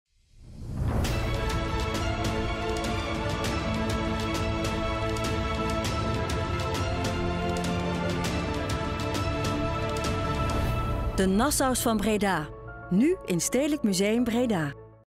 Voice-Over voor commercial Stedelijk Museum Breda 'De Nassau's van Breda'
Voice-Over voor TV Commercial voor het Stedelijk Museum Breda; de tentoonstelling 'De Nassau's van Breda'
Dank @confuegomanagement8417 !